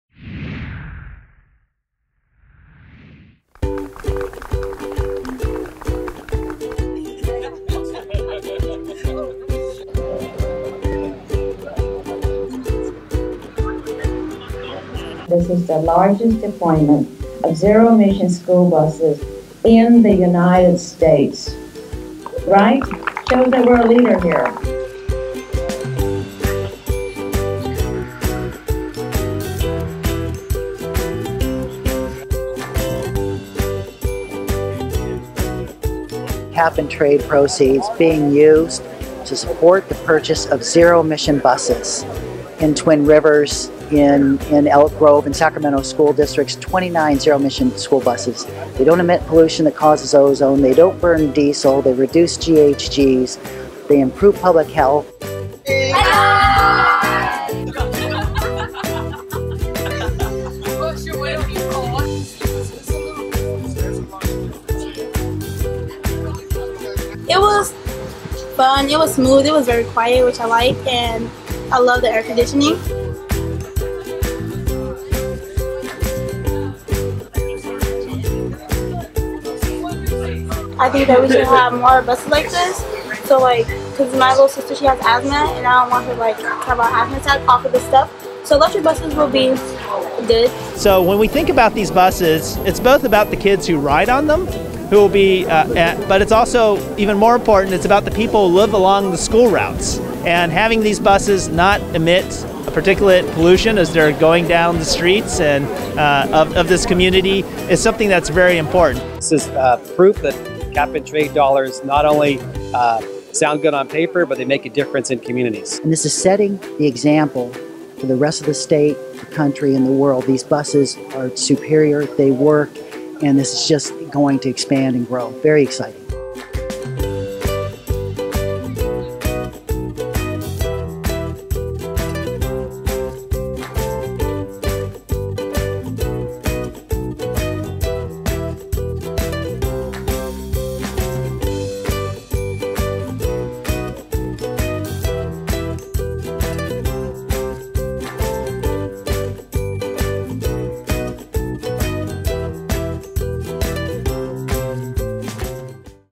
29 zero-emission electric school buses for Sacramento students
The deployment of 29 zero-emission, fully electric school buses was made possible in part by a $7.5 grant from cap-and-trade proceeds and will benefit hundreds of students in the Sacramento, Elk Grove and Twin Rivers Unified School Districts.

It is the largest deployment in the nation!

The “Sacramento Electric School Bus Deployment” is part of the California Climate Investments, a statewide program that puts billions of cap and trade dollars to work reducing greenhouse gas emissions, strengthening the economy and improving public health and the environment.